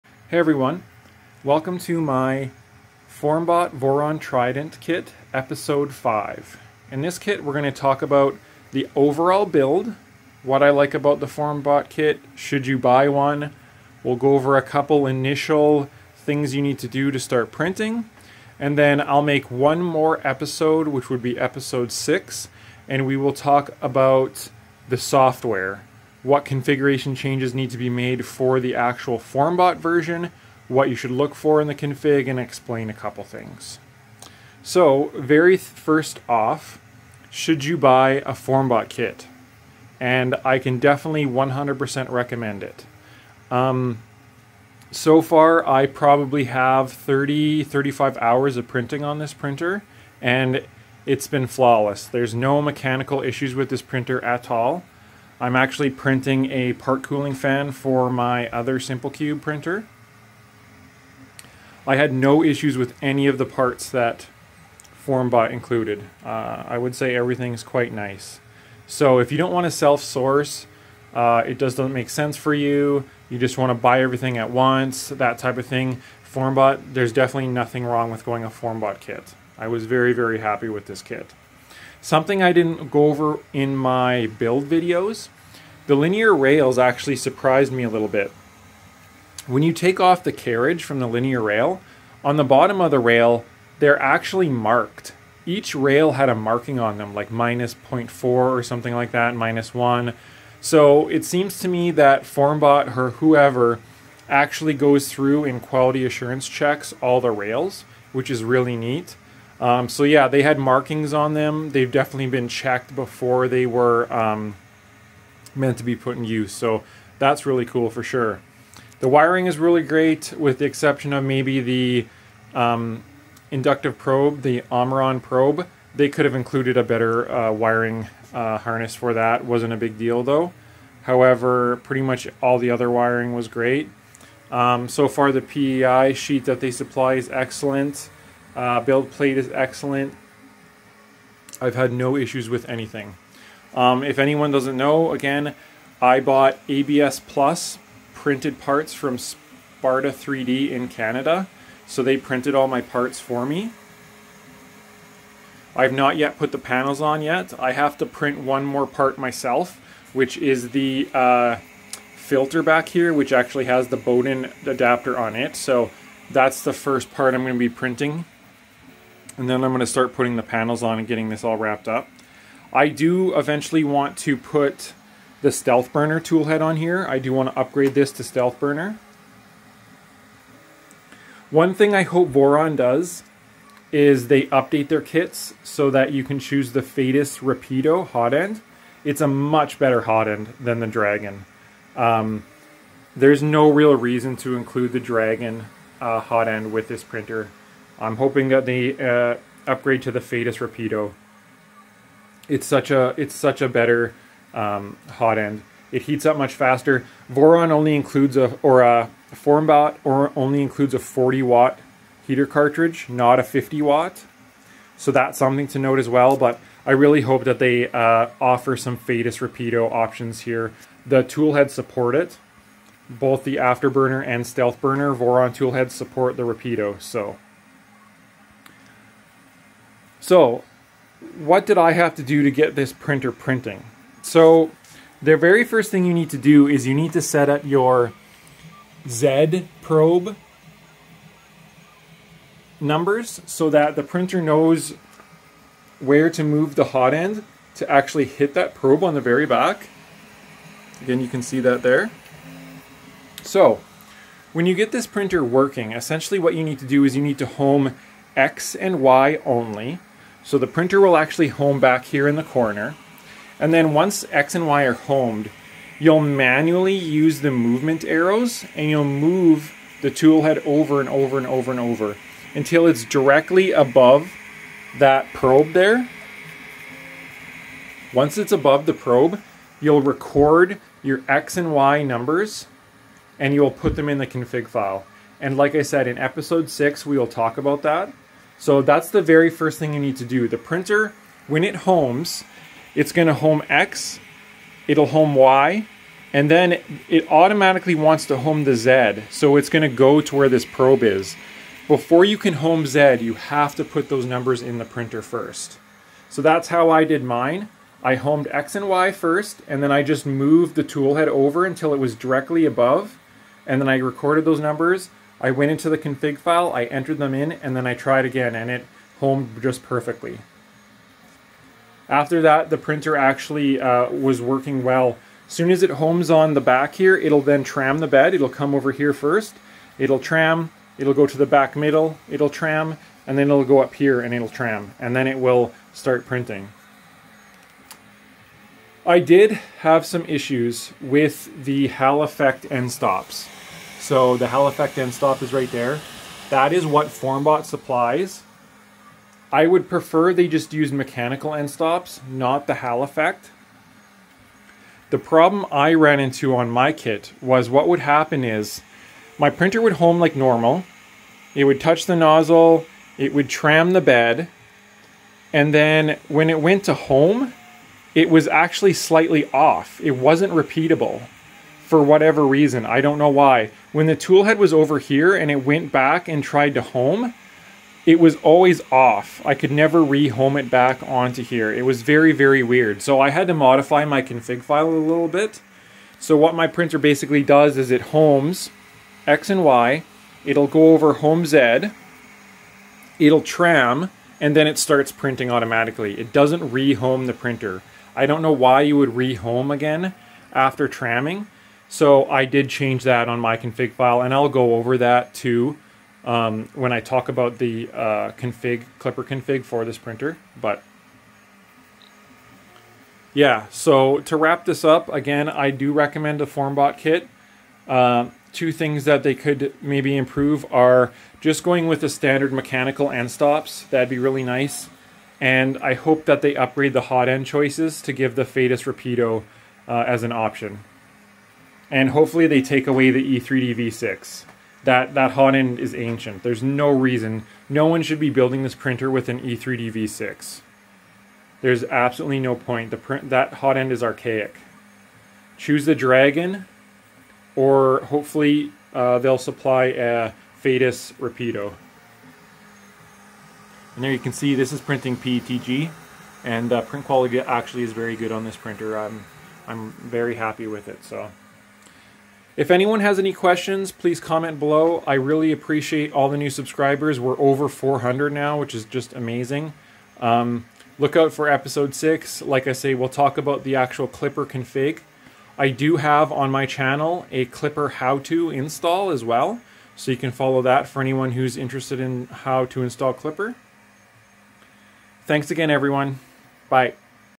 FORMBOT Voron Trident 3D Printer Kit - Episode 5 - Build Review
Here we talk about the final build

Awesome source for 3D printed parts and other 3D Printer stuff!

Here is my new discord server!